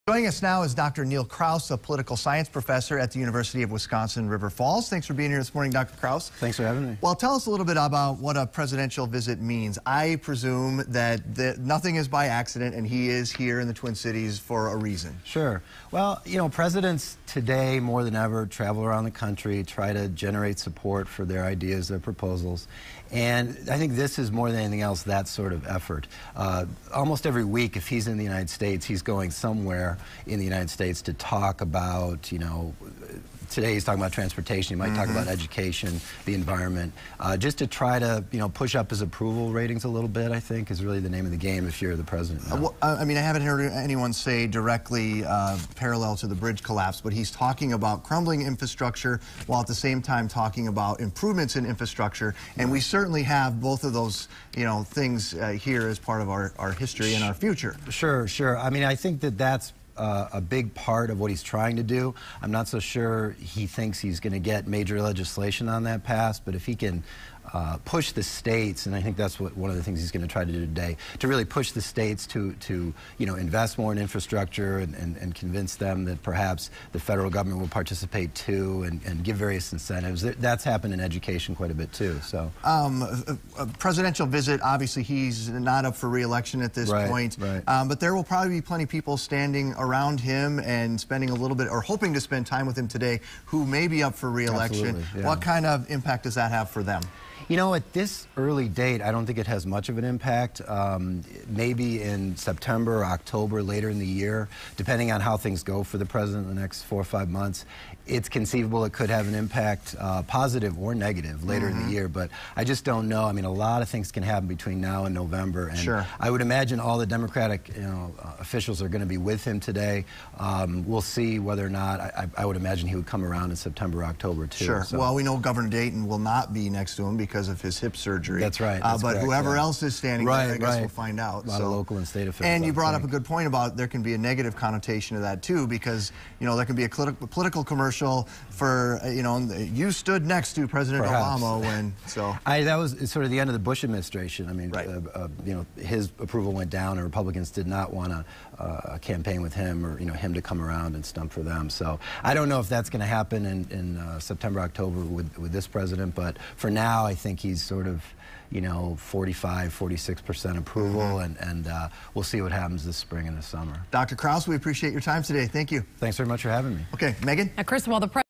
INTERVIEW: Political Expert on President Obama's Visit
President Barack Obama is en route to Minnesota for a visit focused on mass transit. Neil Kraus, A political science professor at the university of Wis.-River Falls joined Chris Eggert in the studio to talk about how the president's visit and speech could effect legislation in the state, transportation projects and elections.